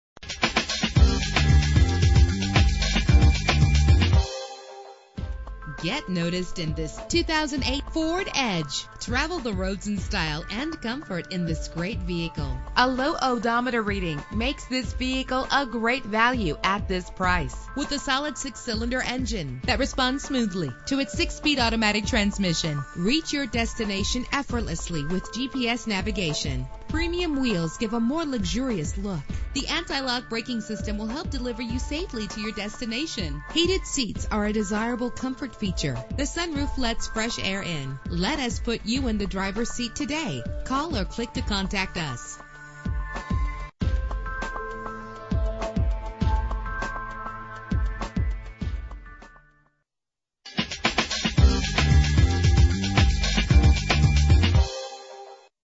2008 Ford Edge - Lancaster PA
Year: 2008
Make: Ford
Model: Edge
Trim: Limited
Engine: 3.5 liter 6 cylinder 24 valve
Transmission: 6-Speed Automatic
Color: Tan
Mileage: 5748
Address:
1020 Lititz Pike